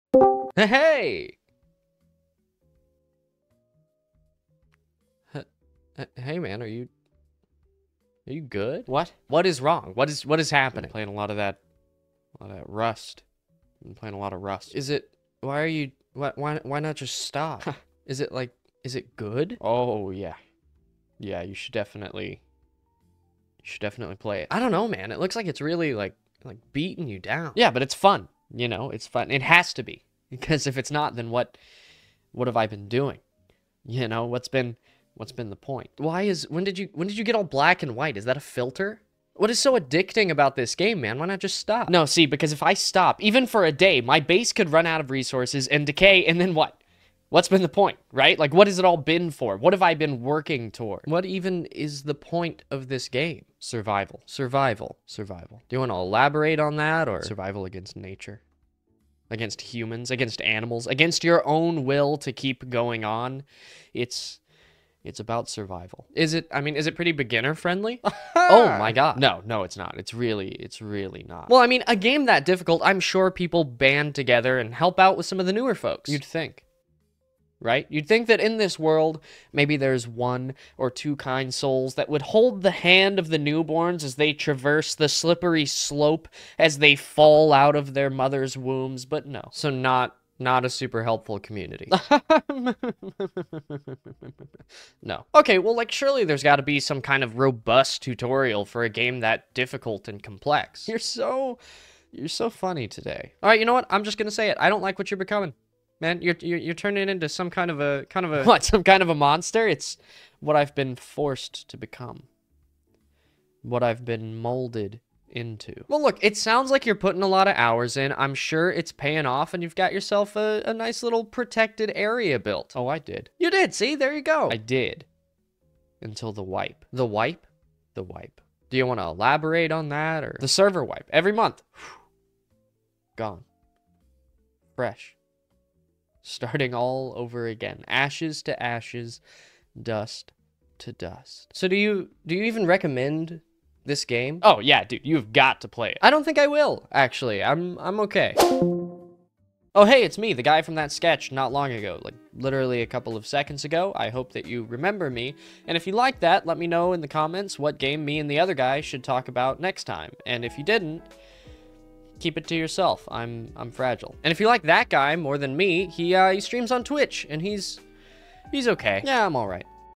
You Should Play: Rust
Just plenty of reasons you should DEFINITELY play Rust, if you just want to... hurt.

This is another game I do genuinely enjoy, and highly recommend to all my fellow masochists.